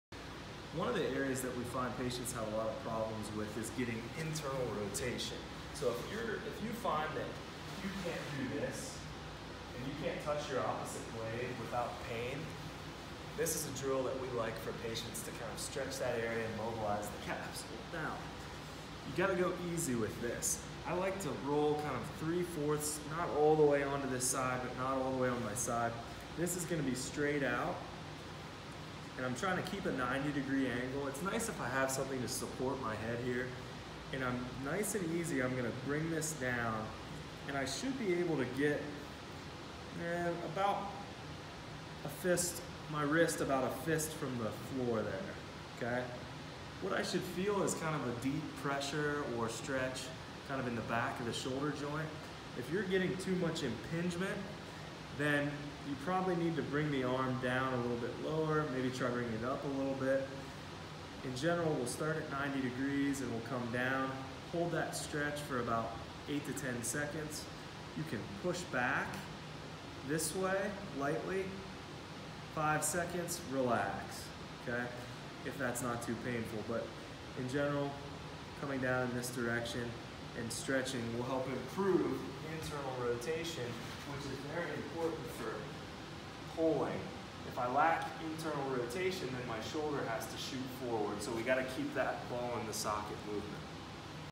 Improve Shoulder Internal Rotation
Commonly referred to as the Sleeper Stretch - this exercise can cause some irritation if not done correctly so pay attention to the details.  However - after your soft tissue/release work this helps work through  an important shoulder internal rotation range of motion.